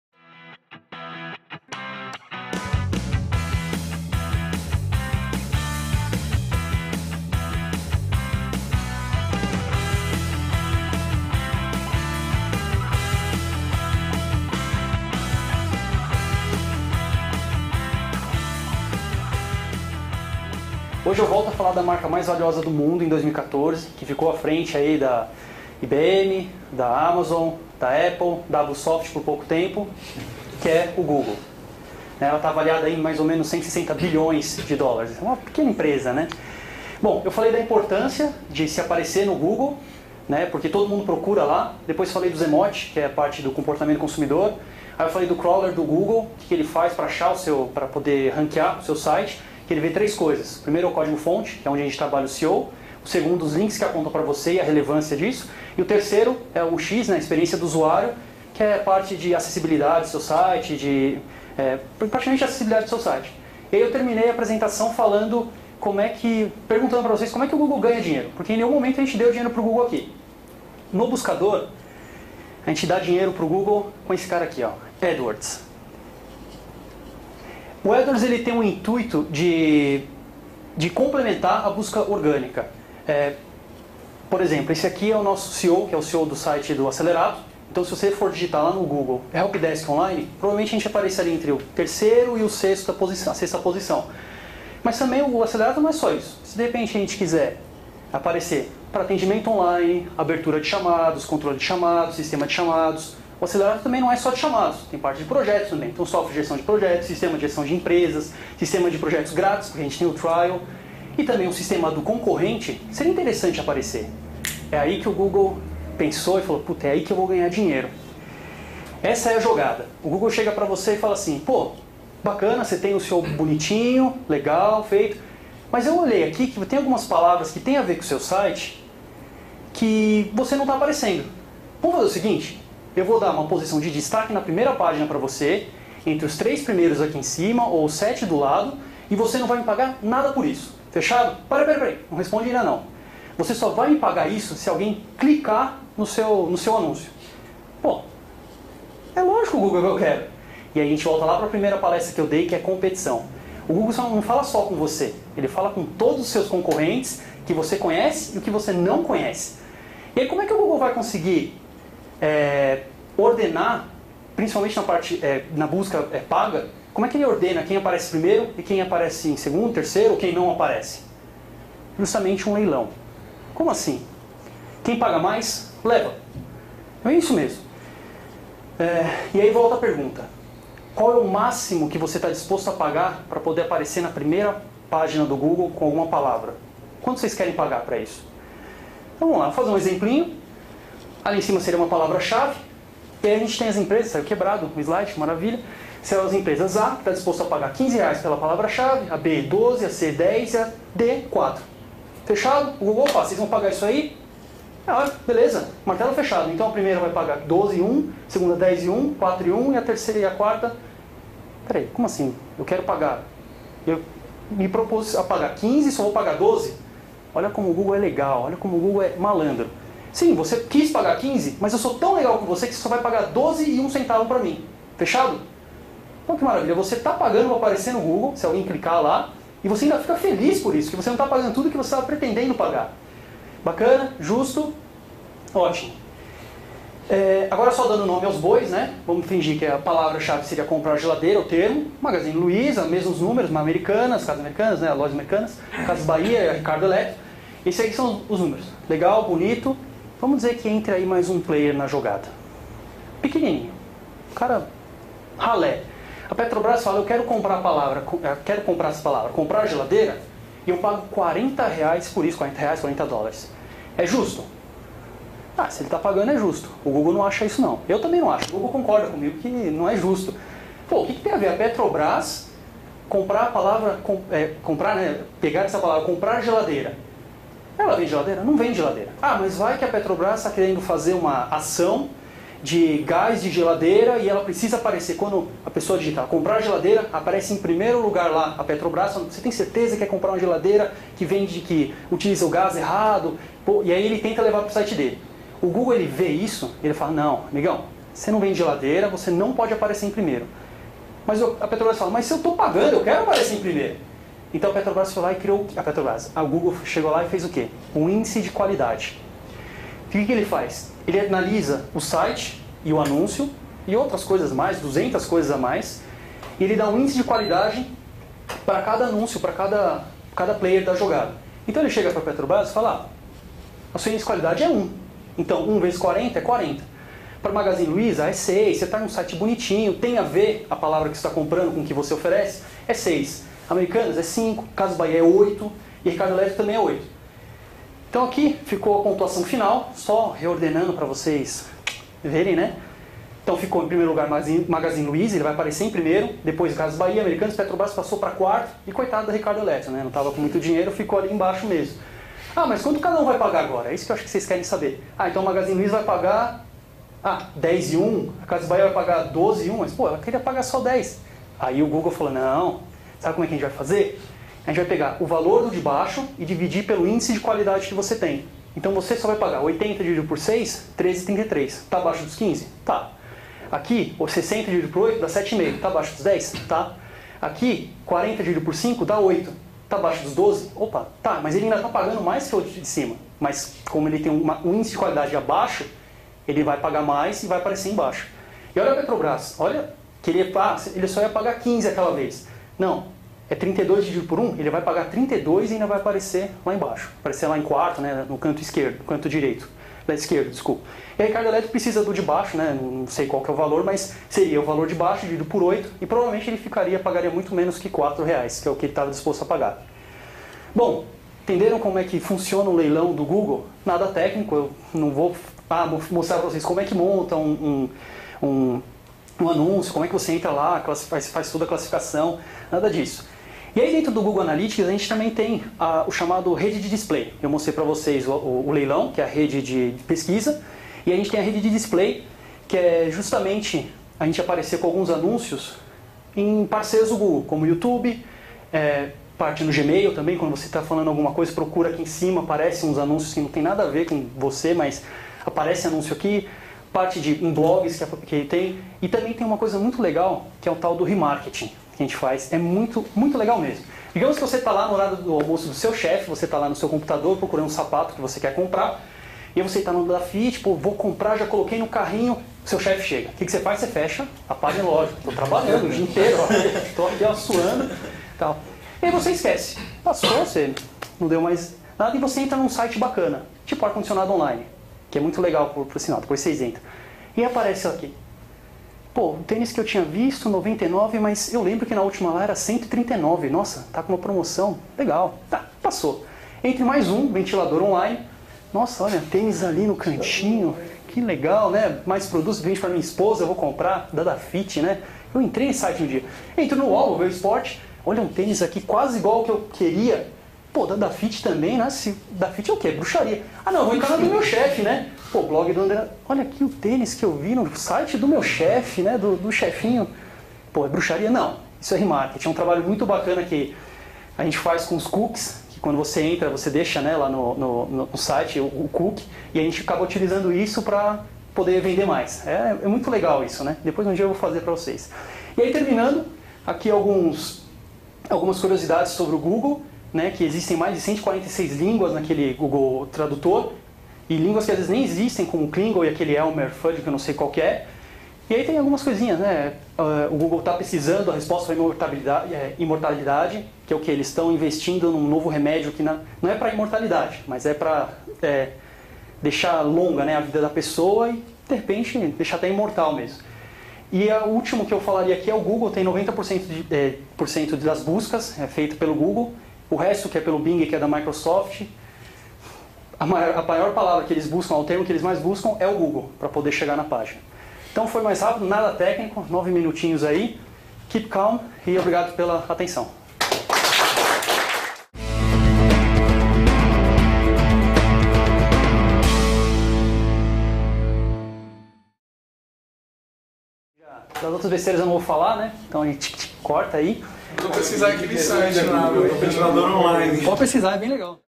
Google Adwords | Wilson Souza | Papo Reto | T1E9
Neste Papo Reto , Wilson Souza palestra novamente sobre o Google.
Nesta segunda parte da apresentação, ele demonstra como funciona o Google Adwords, o leilão, índice de qualidade, rede de pesquisa, display e remarketing.

O intuito não foi explicar a parte técnica do Adword (CPA, CPM, CPC, Anuncio), mas sim um overview de como funciona a parte paga do Google.

Este Papo Reto é um oferecimento de Acelerato - Gestão de Projetos ágeis e Helpdesk

Acesse e descubra como podemos ajudá-lo. Site Bluesoft